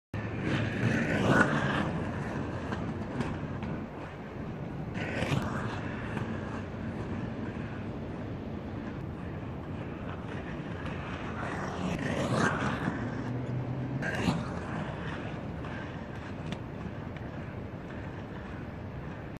Just me, my skates, and some empty streets.
That feeling when you're one of maybe a dozen people awake in the city, so you go for a night blade, taking advantage of the open roads. Also an experiment in shot/edit continuity, this is a zig-zag from Middle St. to Clifford, then to Adams. Only fell once!